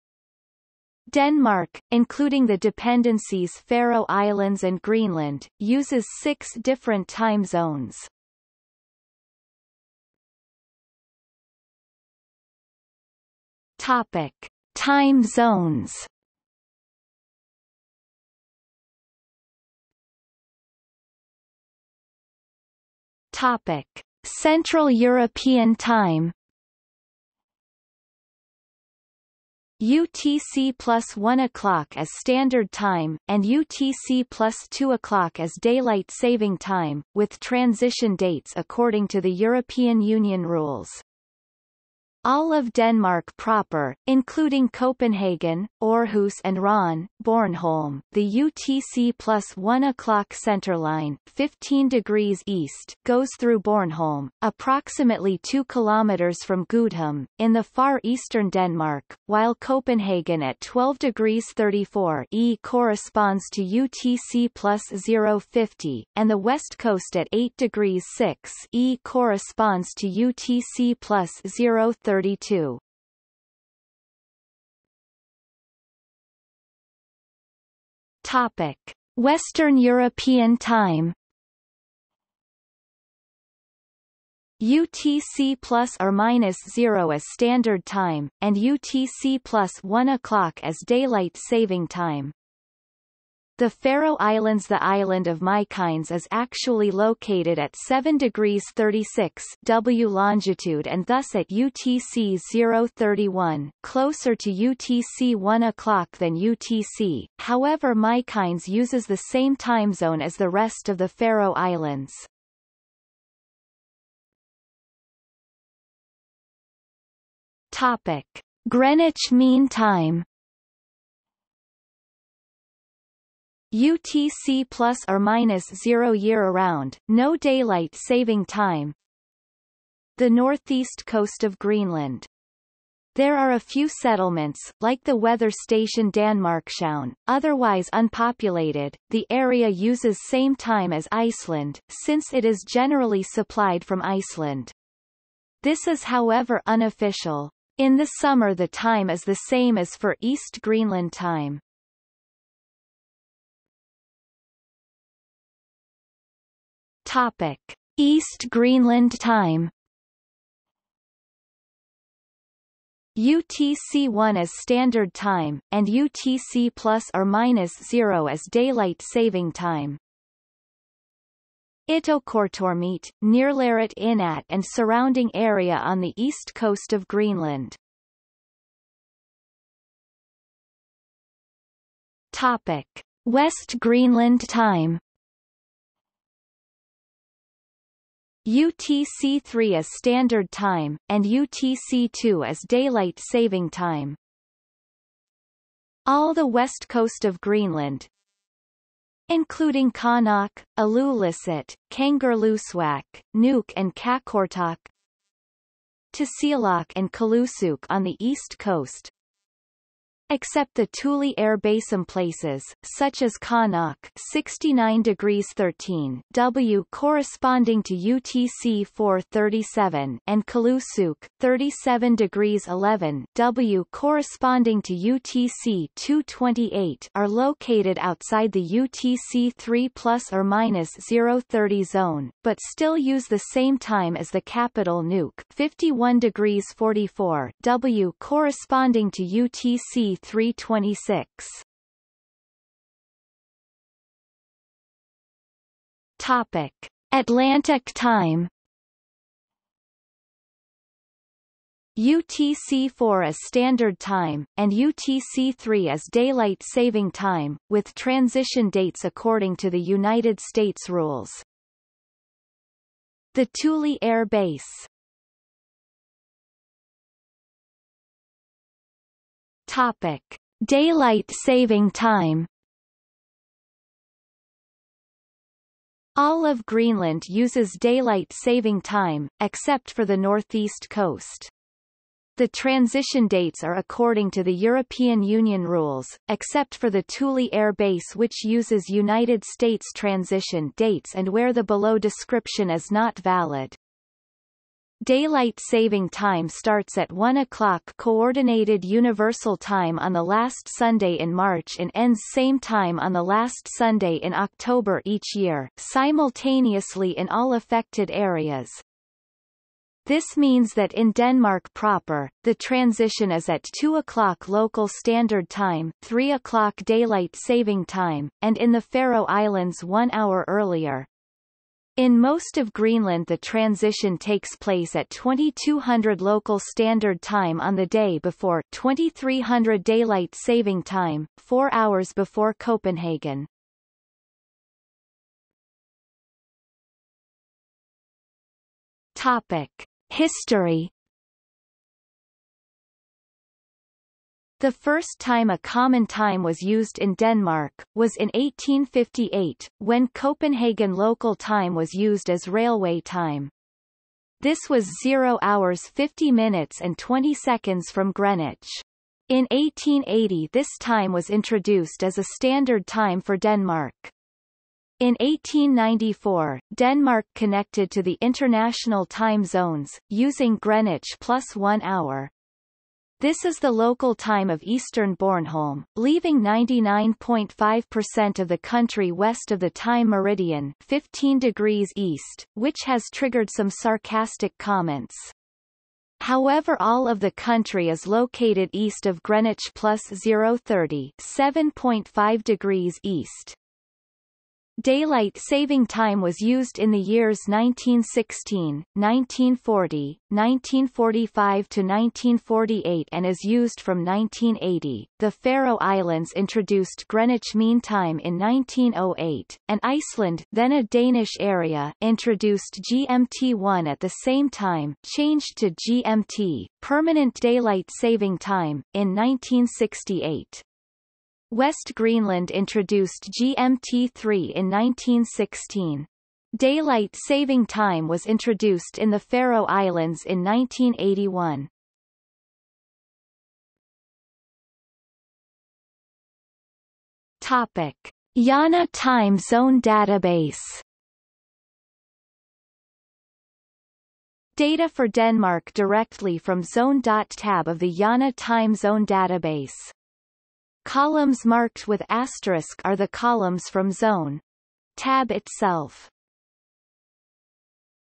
Time in Greenland | Wikipedia audio article
00:00:10 1 Time zones
00:00:19 1.1 Central European Time
00:01:14 1.2 Western European Time
00:01:54 1.3 Greenwich Mean Time
00:02:33 1.4 East Greenland Time
00:02:58 1.5 West Greenland Time
00:04:02 1.6 Atlantic Time
00:04:26 2 Daylight saving time
00:05:35 3 History
00:07:13 4 IANA time zone database

- Socrates

SUMMARY
Denmark, including the dependencies Faroe Islands and Greenland, uses six different time zones.